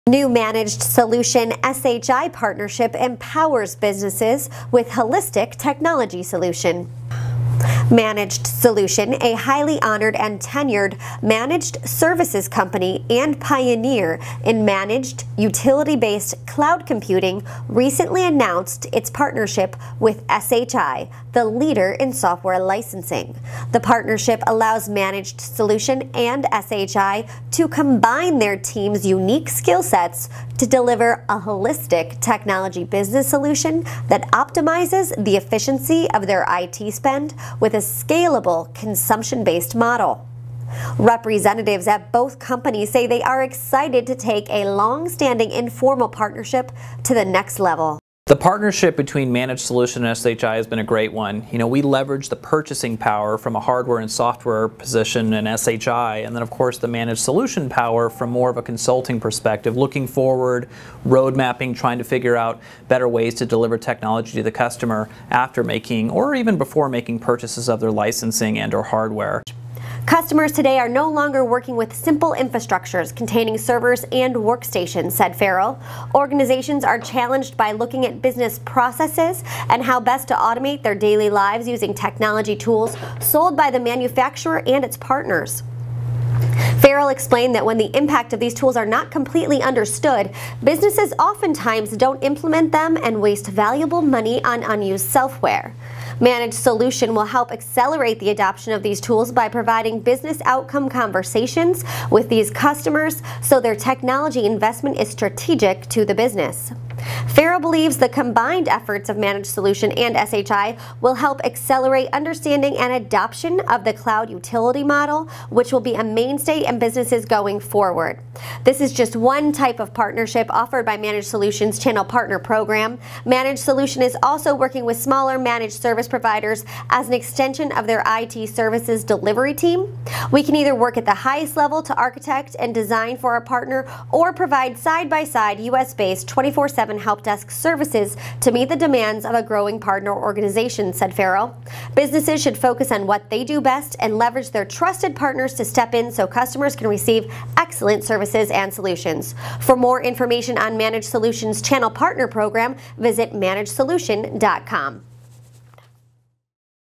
Managed Solution x SHI Partnership - Empowers Businesses with Holistic Technology Solution
Managed Solution, a highly honored and tenured managed services company and pioneer in managed, utility-based cloud computing, recently announced its partnership with SHI, the leader in software licensing. The partnership allows Managed Solution and SHI to combine their teams unique skillsets to deliver a holistic technology business solution that optimizes the efficiency of their IT spend with a scalable, consumption based model.

Representatives at both companies say they are excited to take a long-standing informal partnership to the next level.

“This partnership allows technology business consumers to benefit from the purchasing power and expertise of SHI when it comes to licensing and hardware solutions while providing another level of technology consulting by Managed Solution’s engineering experts so customers can leverage their businesses current technology,” said Sean Ferrel, CEO of Managed Solution. ”With Managed Solution’s support, all-encompassing technology solutions will be more easily understood and adopted by the business decision makers of today.”

Ferrel said Managed Solution will work with SHI and its customer base to create roadmaps for the future where cloud based utility models and operational spends converge. “Customers today are no longer working with simple infrastructures containing servers and workstations,” said Ferrel. “Organizations are challenged by looking at business processes and how best to automate their daily lives using technology tools sold by the manufacturer and its partners.”

Ferrel explained that when the impact of these tools are not completely understood, businesses often times don’t implement them and waste valuable money on unused self-ware.  “Managed Solution will help accelerate the adoption of these tools by providing business outcome conversations with these customers so their technology investment is strategic to the business.”

Ferrel believes the combined efforts of Managed Solution and SHI will help accelerate understanding and adoption of the cloud utility model which will be a mainstay in businesses going forward.

This is just one type of partnership offered by Managed Solution’s Channel Partner Program. Managed Solution is also working with smaller Managed Service Providers as an extension of their IT services delivery team.  “We can either work at the highest level to architect and design for our partner or provide side by side U.S. Based 24/7 helpdesk services to meet the demands of a growing partner organization, said Ferrel.  “Businesses should focus on what they do best and leverage their trusted partners to step in so customers can receive excellent services and solutions.”